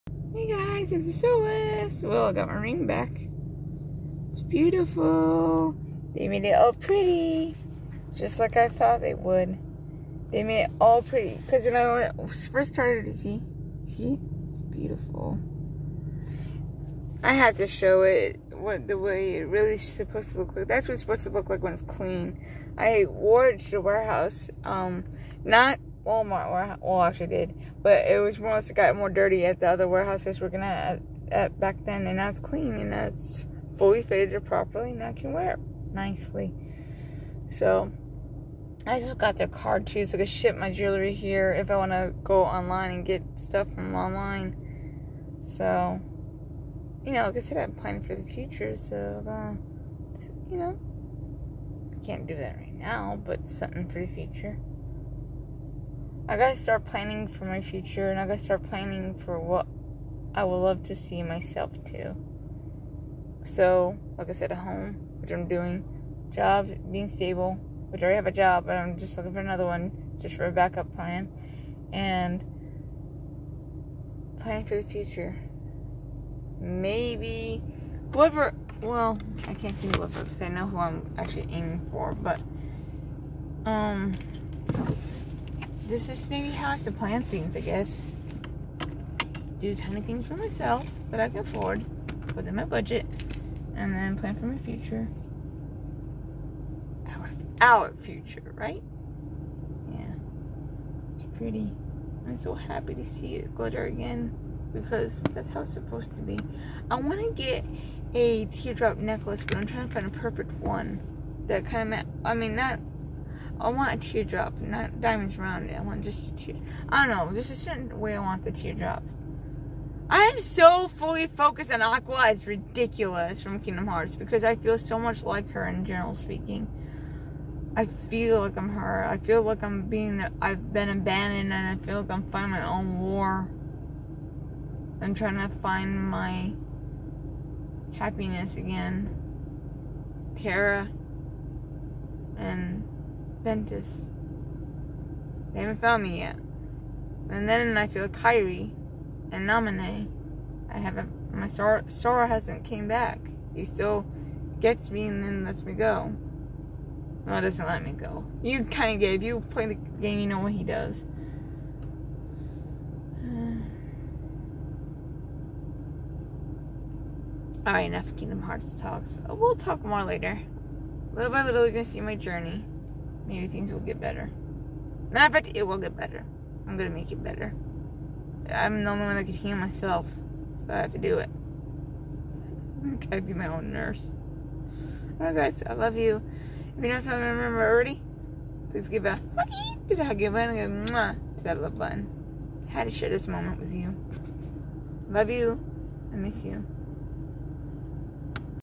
Tear Drop Ring Is back And My Future
DancingSouless Gets her Tear drop Ring back and it fits perfectly on her hand this time with lots of glitter after being nicely cleaned.Mean while she talks about her plans  about her future purchases 😃

Here Is A list To All Special Blogg Letters and InBetween Storys (They all Connect To Eachother)

Skype
AlexiaAdmiral666